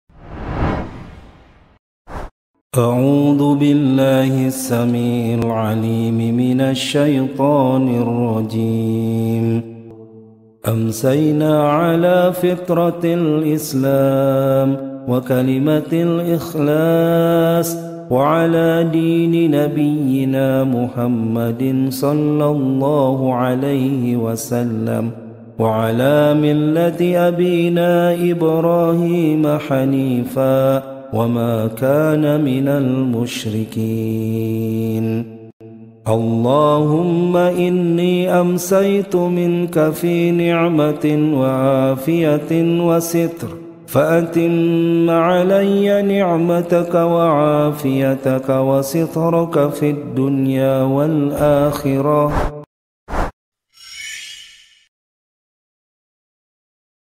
Dzikir Petang
Sahabatku, Rasulullah mengajarkan kepada kita untuk selalu berdzikir. Salah satu dzikir yang diajarkan adalah dzikir petang. Dzikir ini memiliki banyak sekali keutamaan.
Mari kita untuk melaksanakan dzikir petang untuk 1 menit ini. Kita luangkan untuk mengingat Allah yang telah memberikan kenikmatan dan rahmat. Mari kita untuk selalu melakukan kebaikan dan mengingat Allah.
Evening Dhikr
My brothers, Rasulullah teaches us to always do dhikr. One of the dhikr that is taught is the evening dhikr. This dhikr has many virtues.
Let's do the evening dhikr for this 1 minute. We take time to remember Allah who has given us pleasure and mercy. Let us always do good and remember Allah.
Voice of:  Ust. Mulyadi Al Fadhil, M.Pd